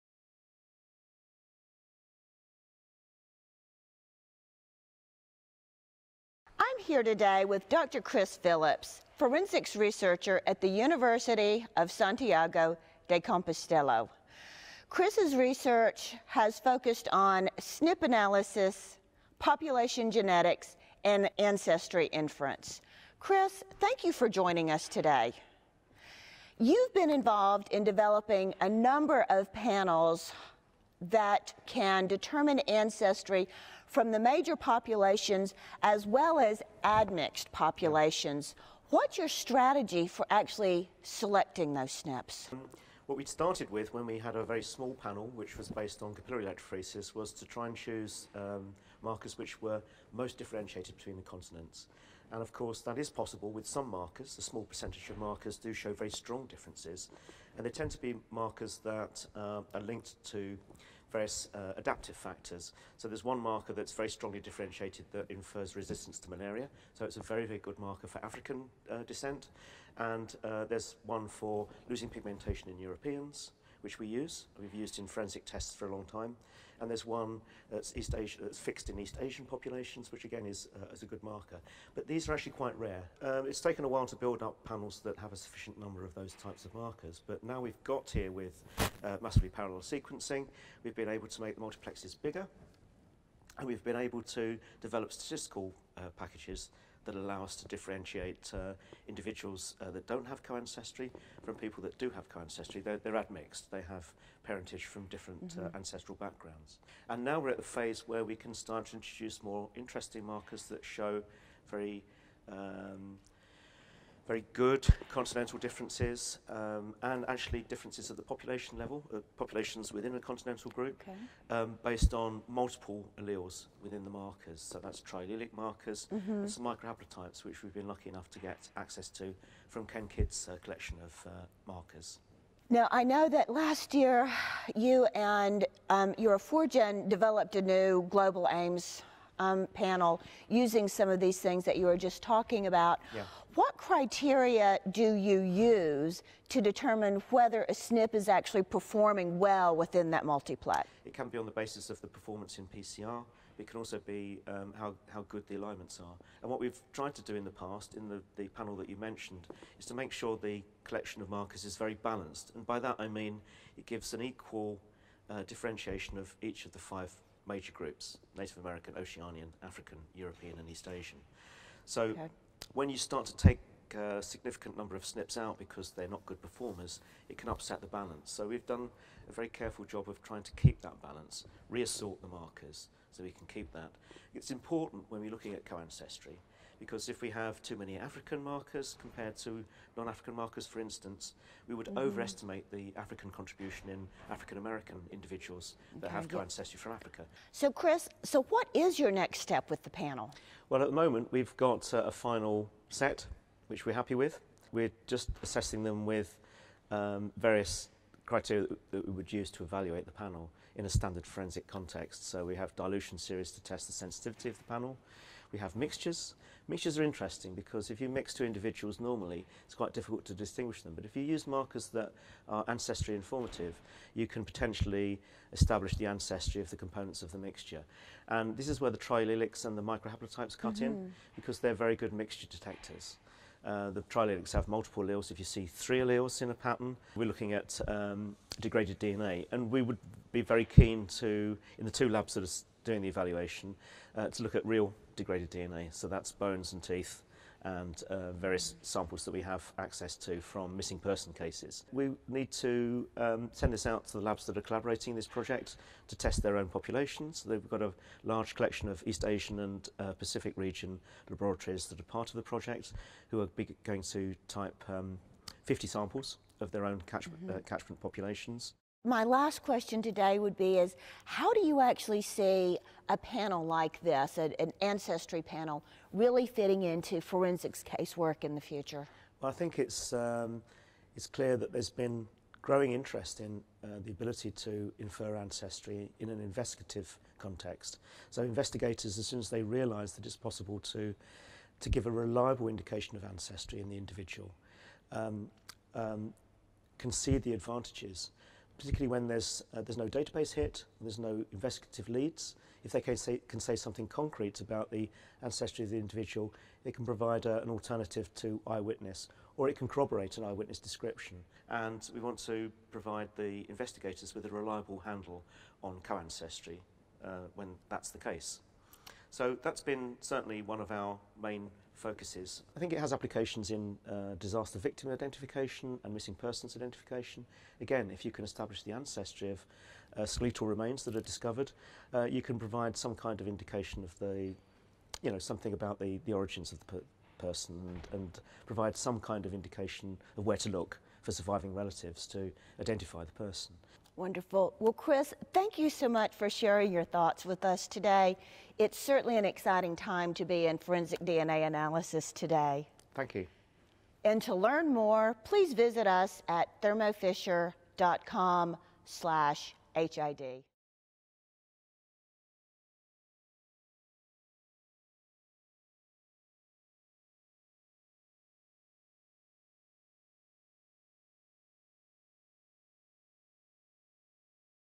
Development of a Custom Ancestry Informative Marker Panel
Dr Chris Phillips, forensics researcher at the University of Santiago de Compostela, focuses on SNP analysis, population genetics and ancestry inference in human identification. Here he discusses strategies to select SNPs that can distinguish the major continental populations as well as admixed population and the criteria he recommends for assessing the effectiveness of a particular SNP.